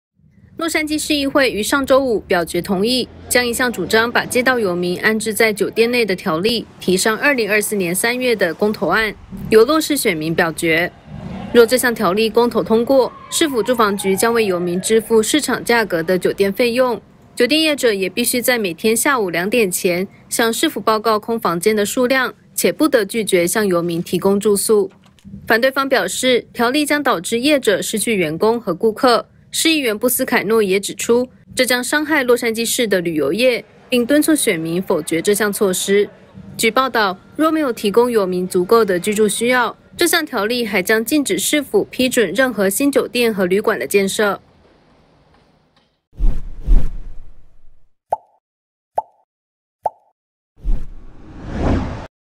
洛市條例強制酒店收容遊民 將於2024年公投｜今日加州
洛市條例強制酒店收容遊民 將於2024年公投

近日，洛杉磯市議會一致同意，將主張把遊民安置在酒店內的條例，提上2024年的公投案，交由選民決定。帶您了解。

洛杉磯市議會於上週五（5日），表決同意，將一項主張把街道遊民安置在酒店內的條例，提上2024年3月的公投案，由洛市選民表決。

若這項條例公投通過，市府住房局將為遊民支付市場價格的酒店費用。酒店業者也必須在每天下午兩點前，向市府報告空房間的數量，且不得拒絕向遊民提供住宿。

反對方表示，條例將導致業者失去員工和顧客。市議員布斯凱諾也指出，這將傷害洛杉磯市的旅遊業，並敦促選民否決這項措施。

據報導，若沒有提供遊民足夠的居住需要，這項條例還將禁止市府批准任何新酒店和旅館的建設。